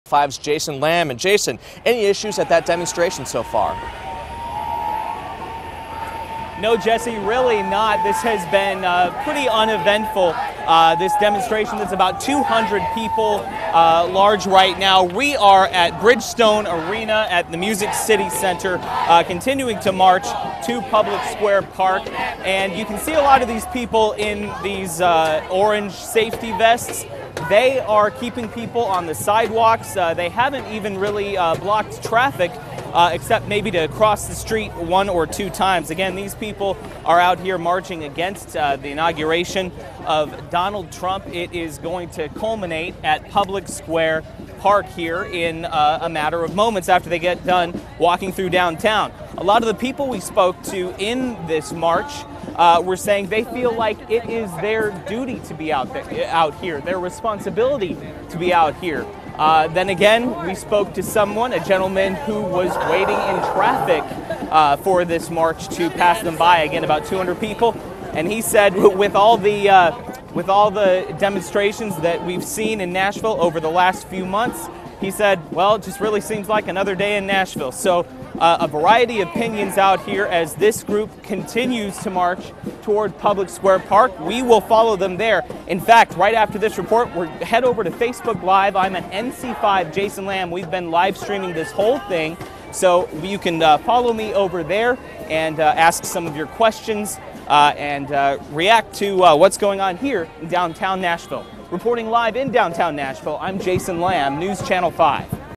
Inauguration Protesters March Through Downtown Nashville
Protesters took to downtown Nashville in opposition of President Donald Trump's inauguration.